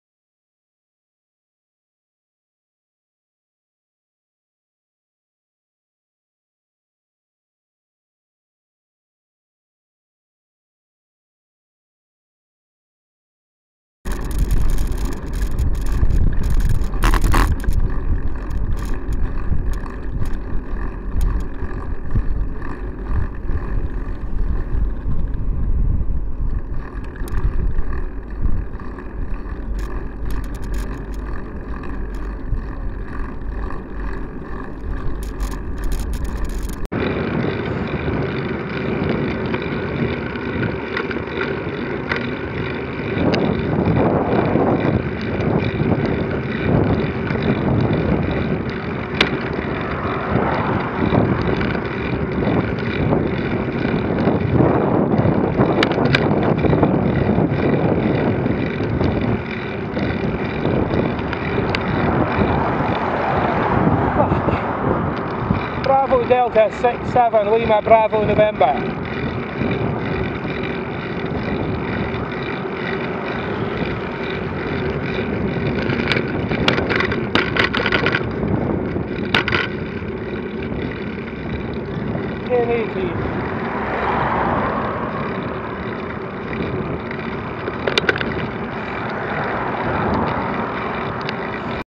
BD67LBN close pass oncoming traffic
Elmdale Avenue, where BD67LBN joins Foleshill road, is a no right turn which is clearly signed.

Sunday 2nd August 2020